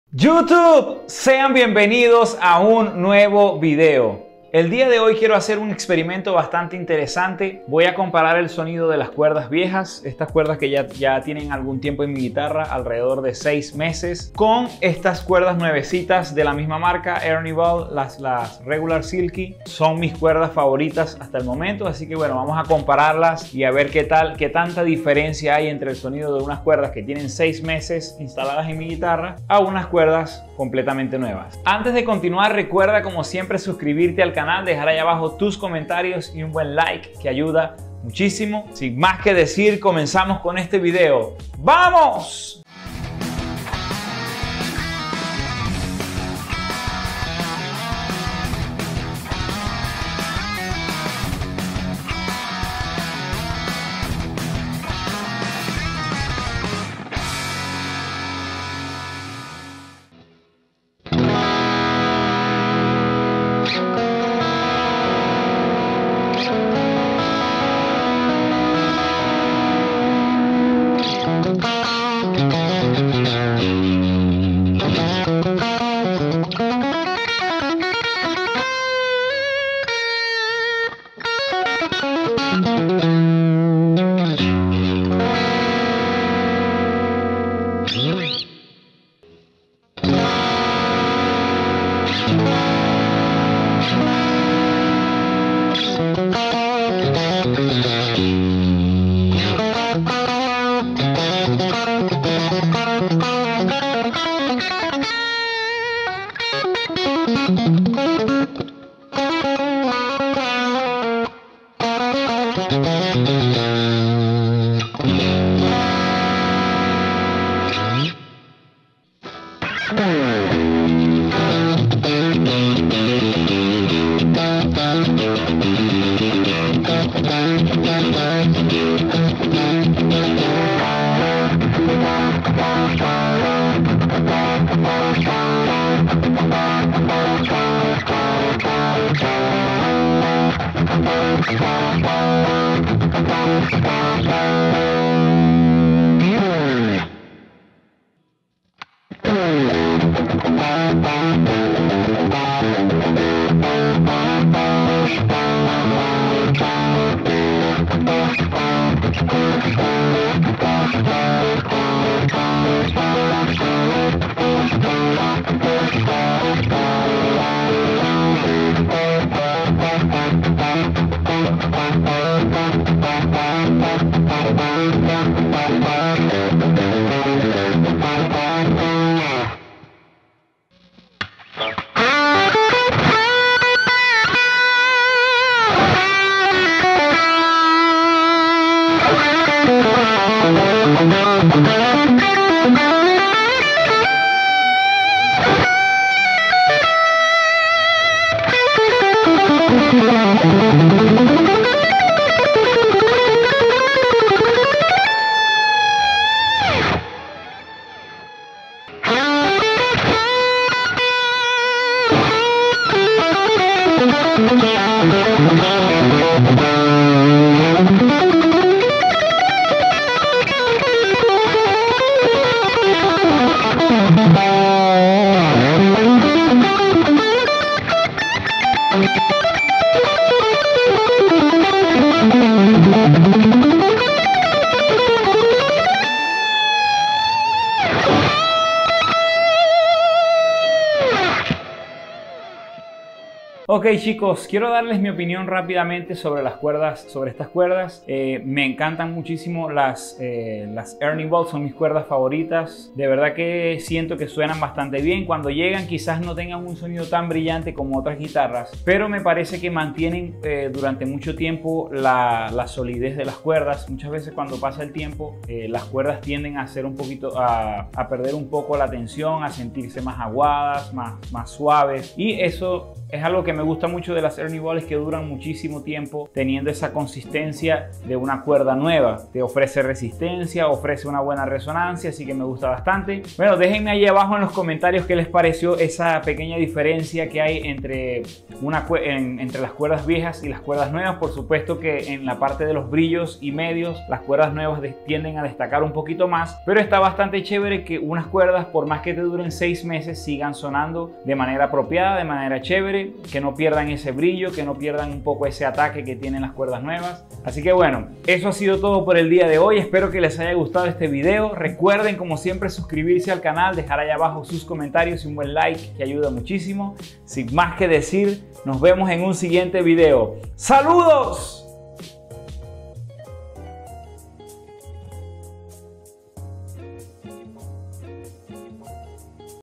Cuerdas nuevas vs Cuerdas viejas l Realmente hay tanta diferencia entre un sonido y otro?
Deja en los comentarios tu opinión acerca de este tema! :D realmente es tan importante tener unas cuerdas nuevas cada 3 meses como se recomienda siempre? o no ay tanta diferencia de sonido?